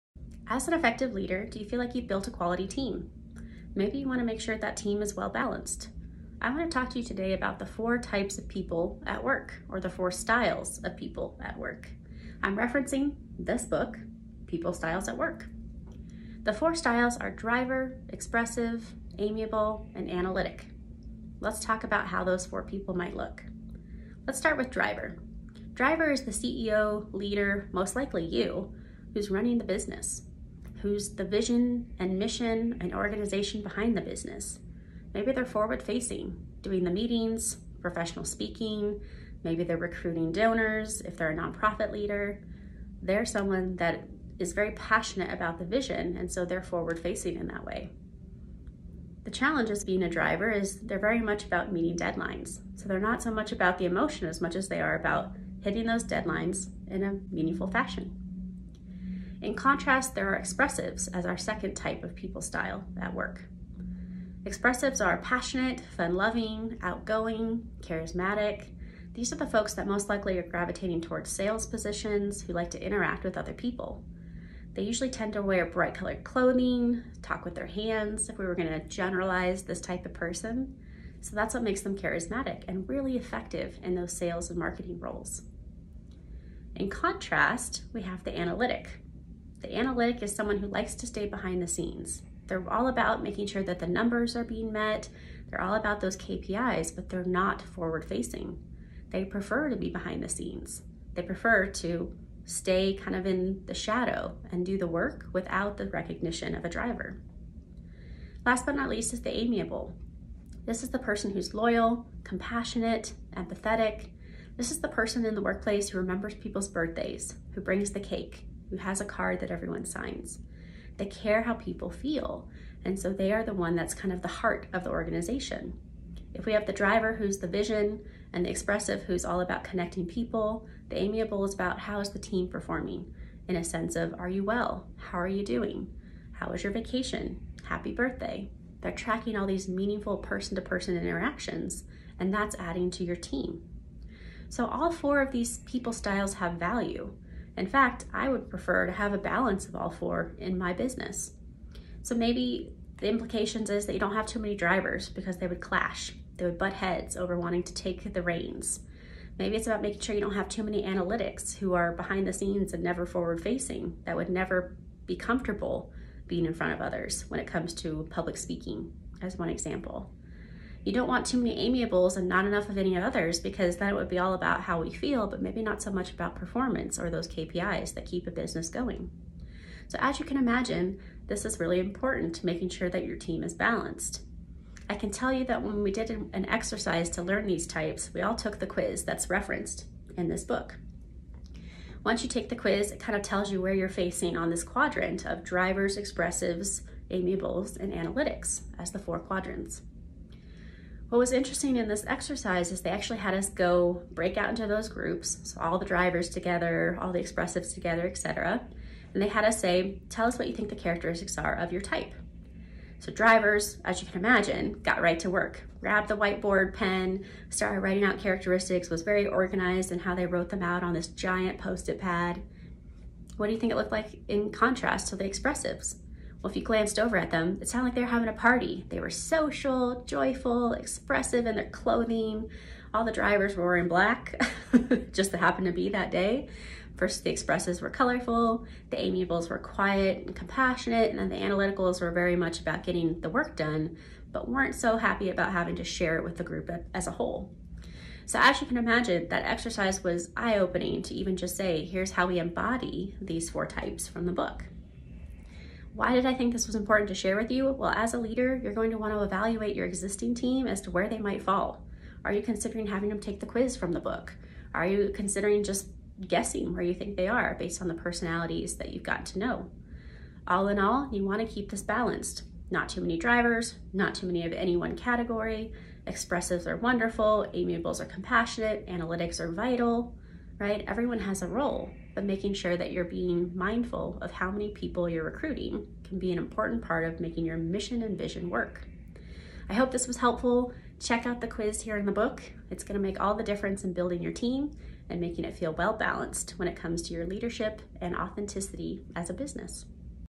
Intentional Leadership and People Styles at Work
Are you curious about the work styles of people at work? Access another tool that can help you build a cohesive team.

Resource: Book People Styles at Work by Bolton & Bolton